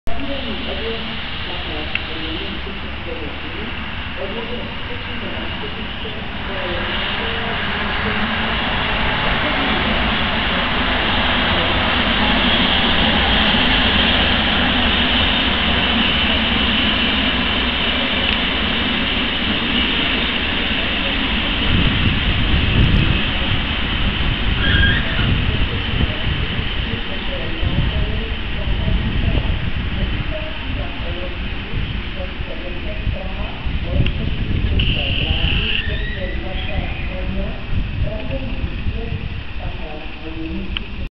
Reko Brejlovec, motorová lokomotiva řady 750.7
Odjezd rychlíku v čele s remotorizavanou lokomotivou ze stanice Zábřeh na Moravě směr Jeseník, únor 2012.